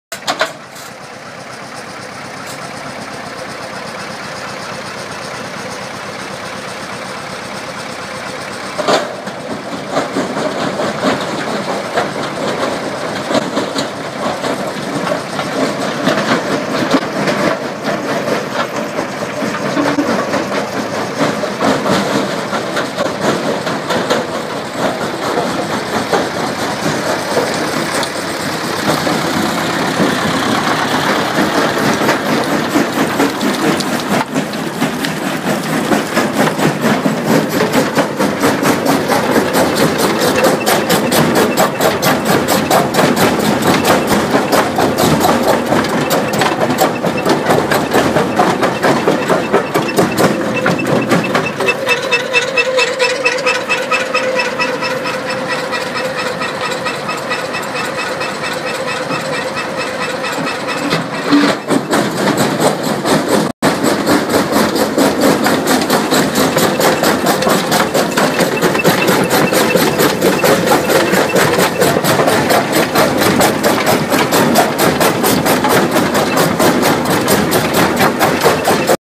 CDI C60 Concrete Sleepers
Just for trials we tried Crushing large concrete Sleepers with great success.
 As you all can see care must be taken when loading to avoid bridging.